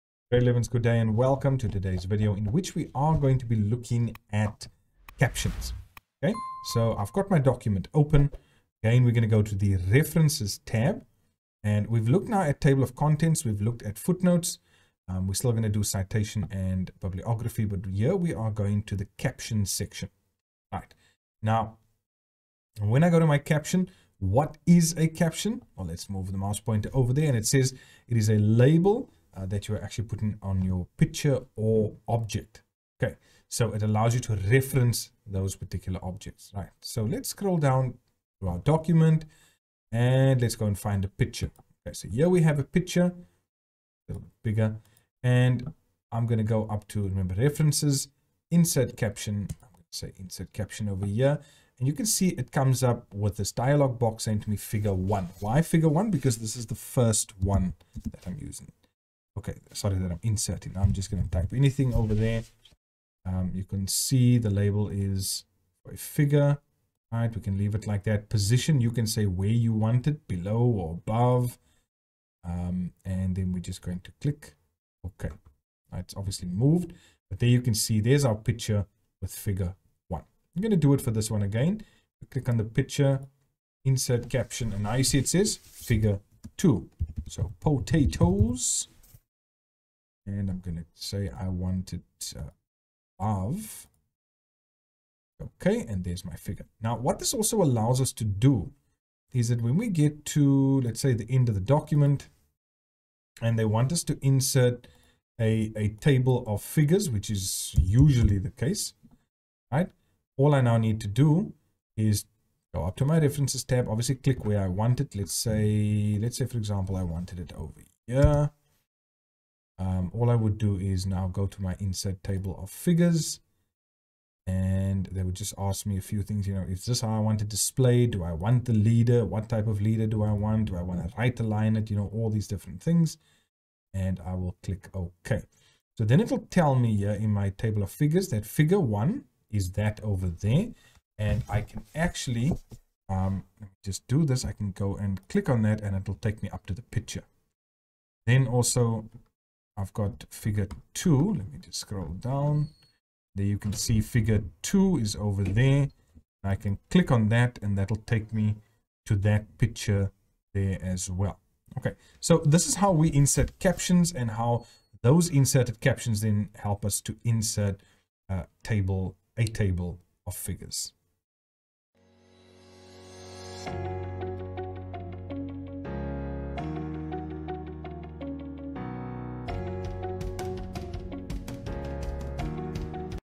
How to Insert Captions in Word
Let's take a quick look at how we can insert captions around our pictures and eventually turn it into a Table of Figures.